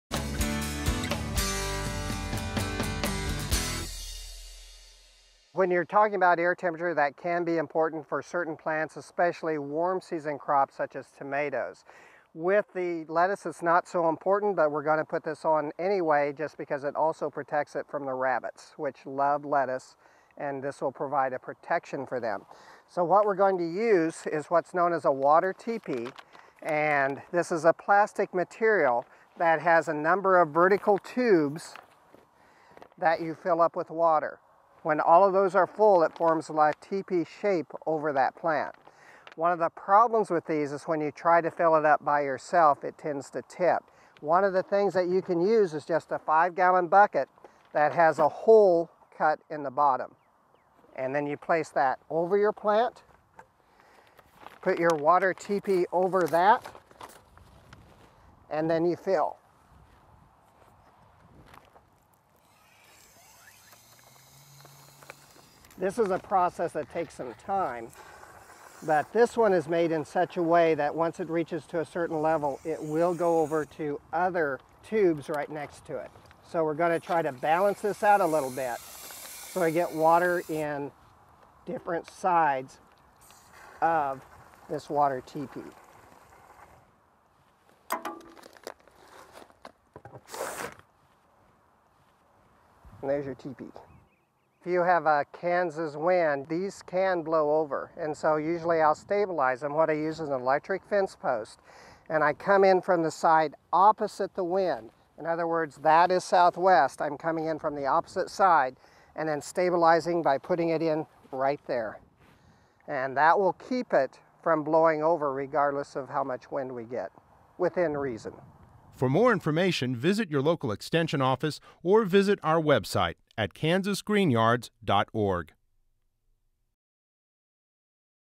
Water Teepees Protect Plants
Water filled teepees store heat during the day, and protects your plants at night from cold temperatures.  This means that you can plant warm season crops such as tomatoes earlier than usual -- and harvest earlier, too!  Produced by the Department of Communications at Kansas State University.